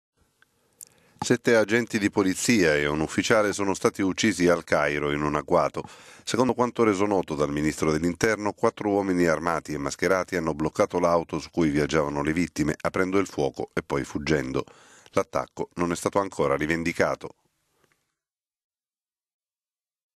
Egitto, agguato a sud del Cair, uccisi sette agenti di polizia e un ufficiale
Sette agenti di polizia e un ufficiale sono stati uccisi al Cairo in un agguato. Secondo quanto reso noto dal ministro dell'Interno, quattro uomini armati e mascherati hanno bloccato l'auto su cui viaggiavano le vittime aprendo il fuoco e poi fuggendo. L'attacco non è stato ancora rivendicato.…